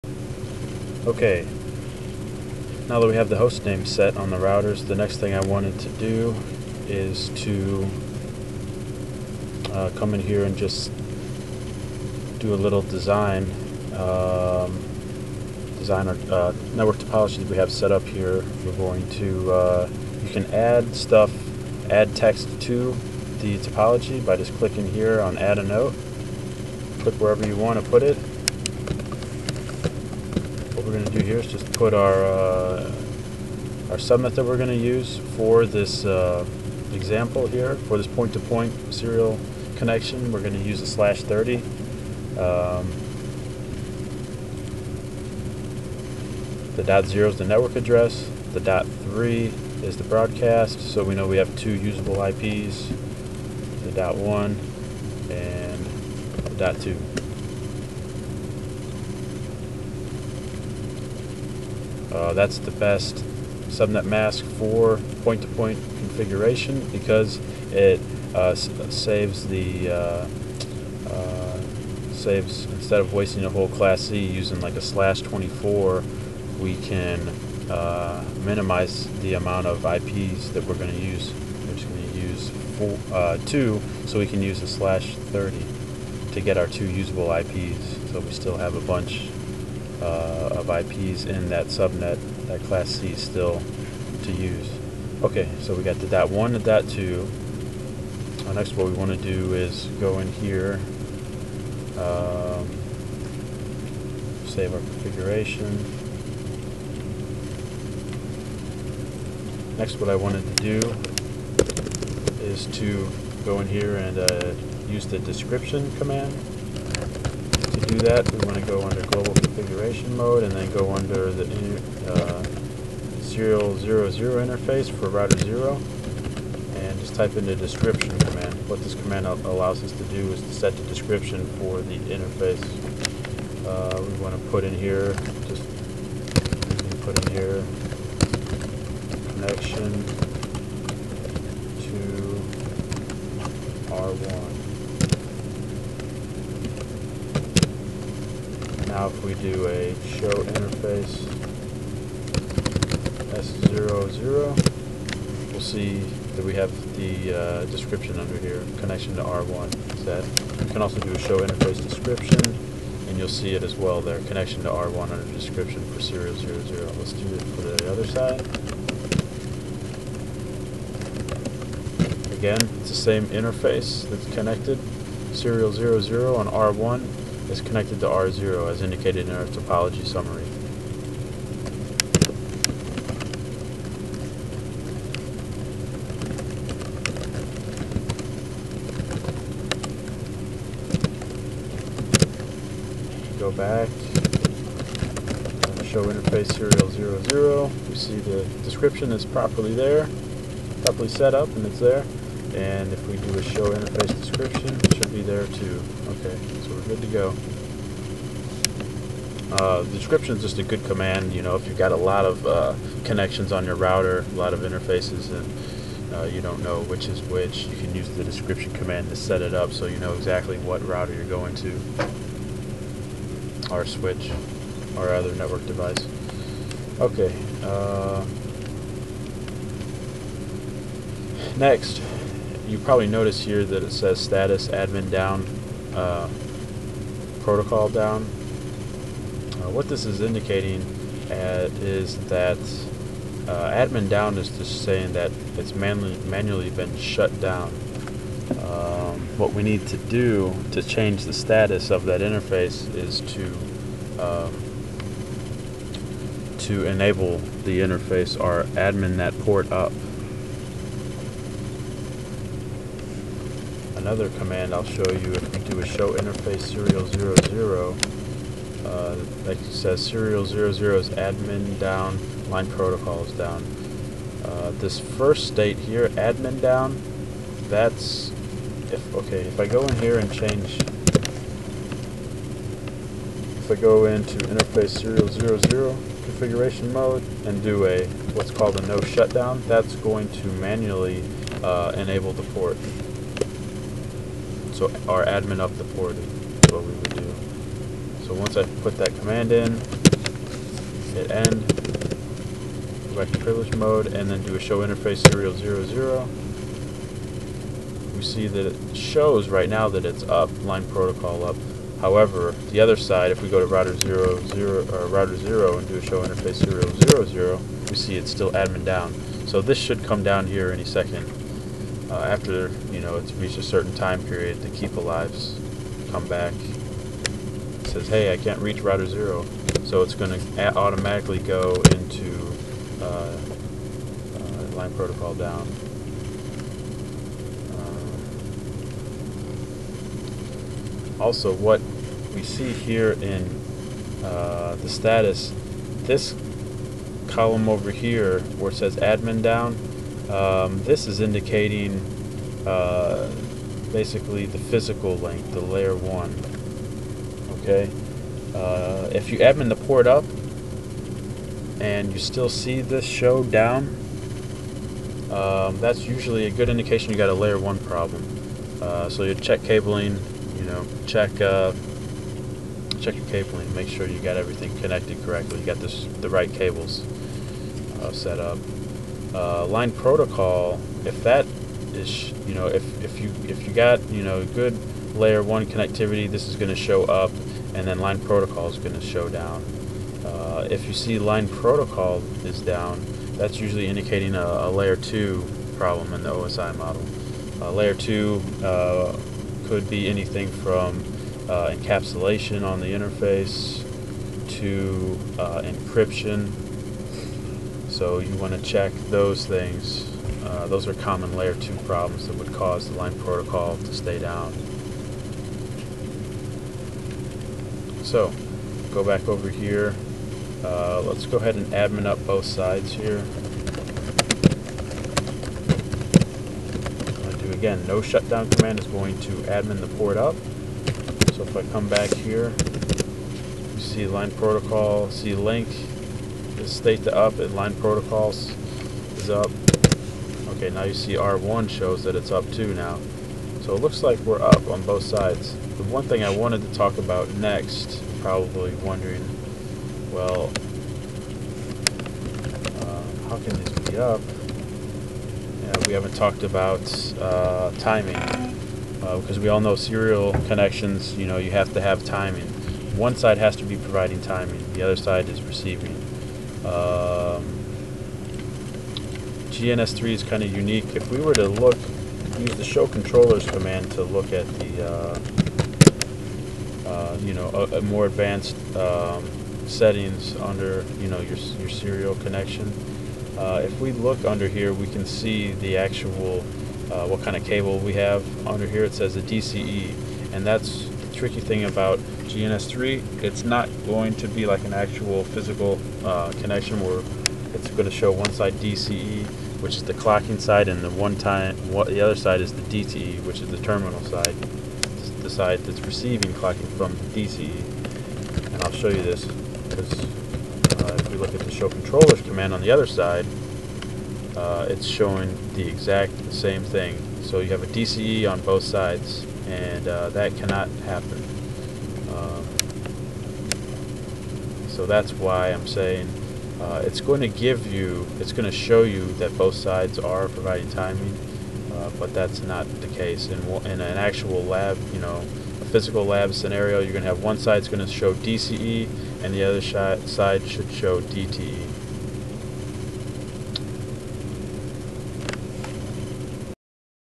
GNS3 INTRO LAB PART 3
This video is my first video in which I describe and Show you how to use the basic's of the GNS3 program. The GNS3 program is a virtual router program that was created to allow networking professionals to practice advanced networking labs without having to buy thousands of dollars of real equipment. I also show you how to setup the following: Idle PC Value, Build a simple Point to Point serial connection between 2 7200 series cisco routers and load the Cisco IOS Image's into GNS3.